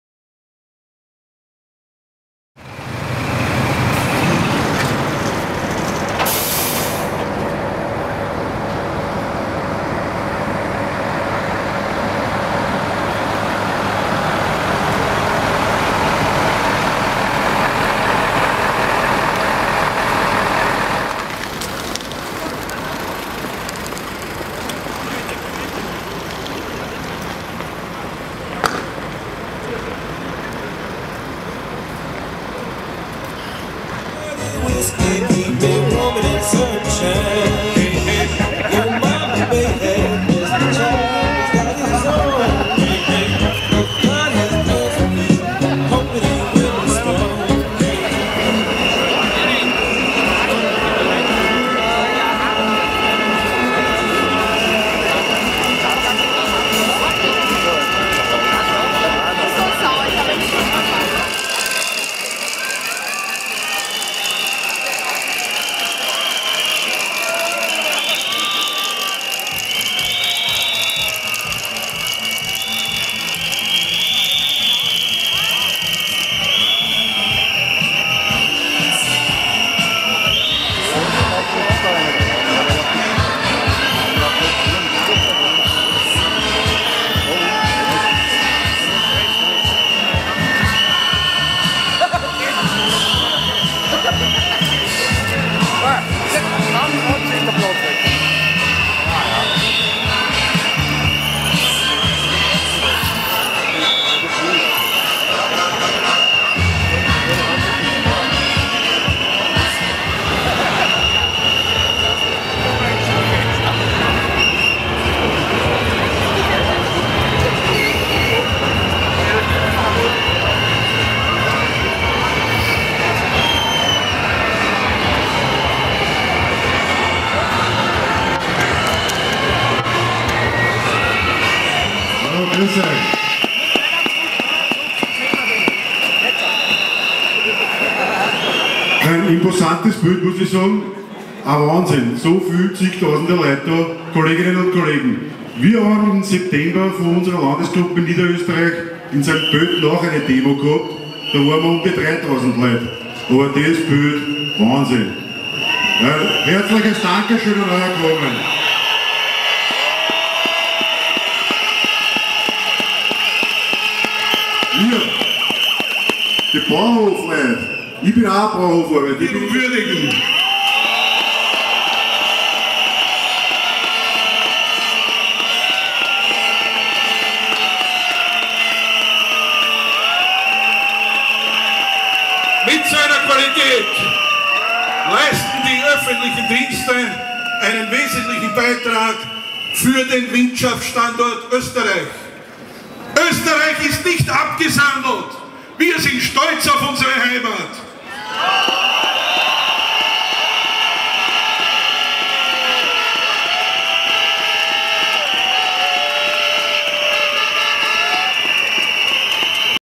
Demo der Österreichischen Bundesbediensteten in Wien (S/W)
Auch Salzburgs Bundesbedienstete machen sich auf den Weg nach Wien. FMT-Pictures berichtet mit Bildmaterial aus Salzburg und Wien.

Organisiert wurde die Kundgebung vor dem Bundeskanzleramt von der Gewerkschaft Öffentlicher Dienst (GÖD) gemeinsam mit der Gewerkschaft der Gemeindebediensteten (GdG).

In der letzten Verhandlungsrunde am vergangenen Dienstag hatte Beamten-Ministerin Gabriele Heinisch-Hosek (SPÖ) ein Prozent plus eine Einmalzahlung von 0,5 Prozent angeboten. Die Gewerkschaft beharrt zumindest auf einer Abgeltung der Inflation von 2,3 Prozent.

Neben den Gehaltsverhandlungen protestieren die Beamten aber auch gegen den Bruch der sozialpartnerschaftlichen Gepflogenheiten durch die Regierung beim Beschluss des Lehrerdienstrechtes sowie die angespannte Personalsituation in vielen Bereichen in Folge des fortgesetzten Aufnahmestopps.